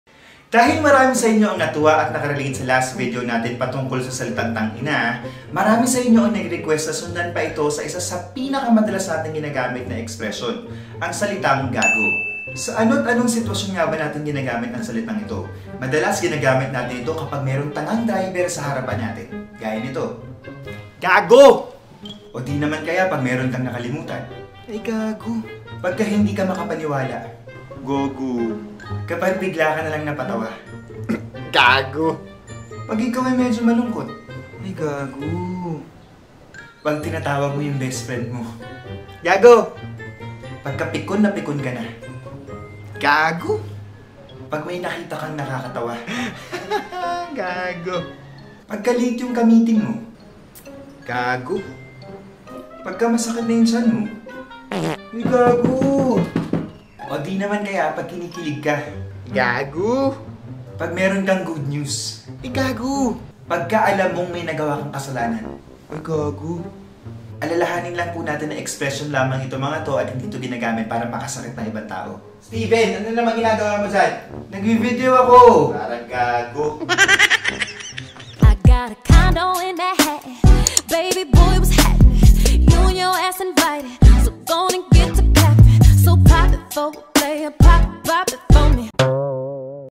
Gamit ng salitang GAGO | Steven Bansil
Ano ano nga ba ang iba't ibat gamit ng salitang "GAGO"?

That's all I need to keep going with my videos and my channel.

Stay crazy! =)